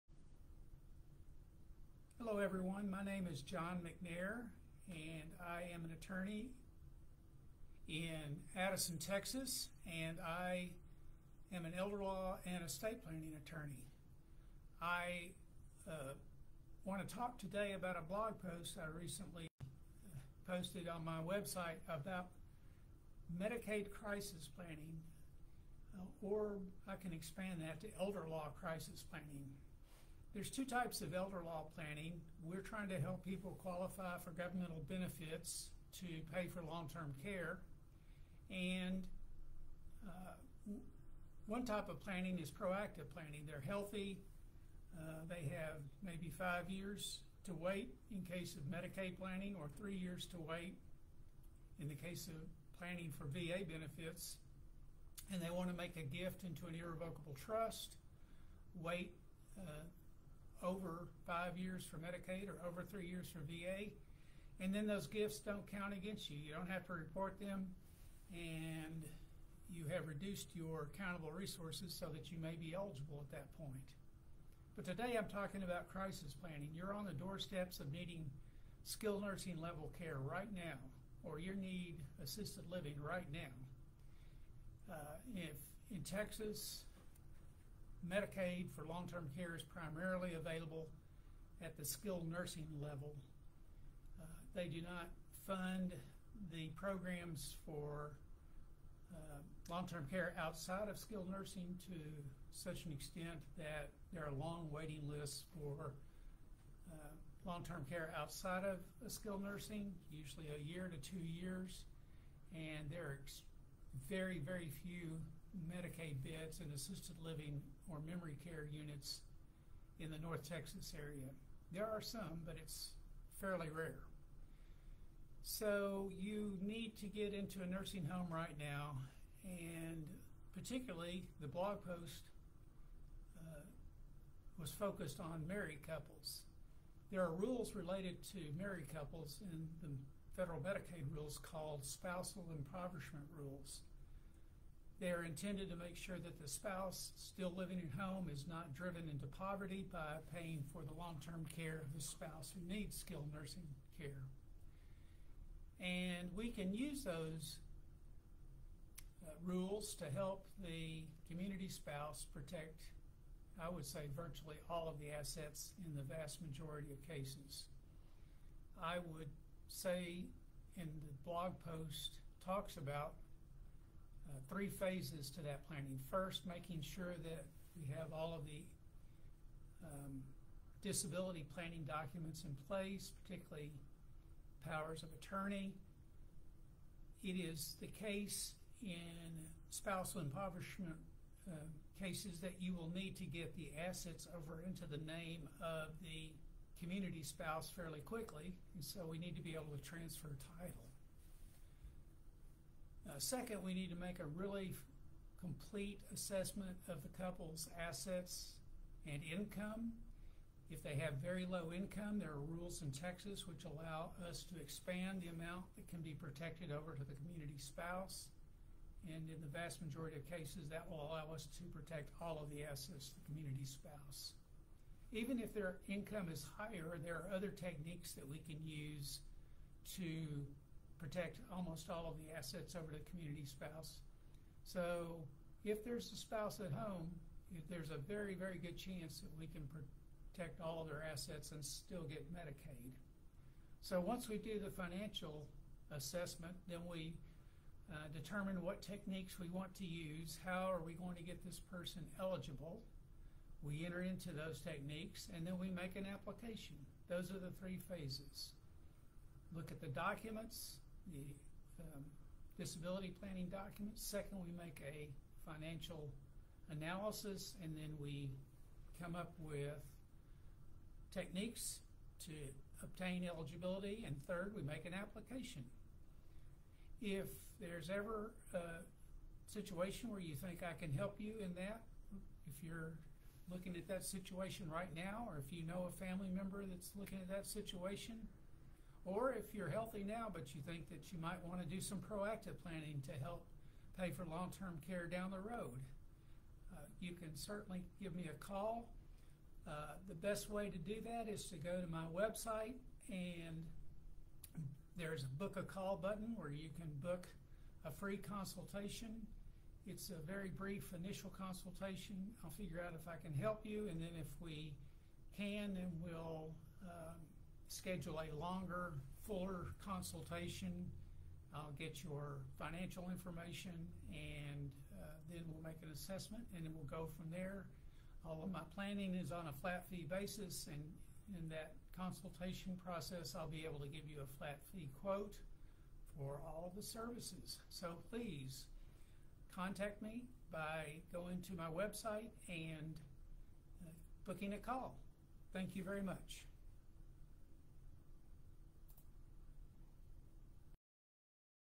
Texas Medicaid Planning Essentials * McNair Dallas Law
Dallas Certified Elder Law Attorney John McNair describes essential strategies for Texas Nursing Home Medicaid and Veteran's Administration Pension (Aid & Attendance) crisis planning.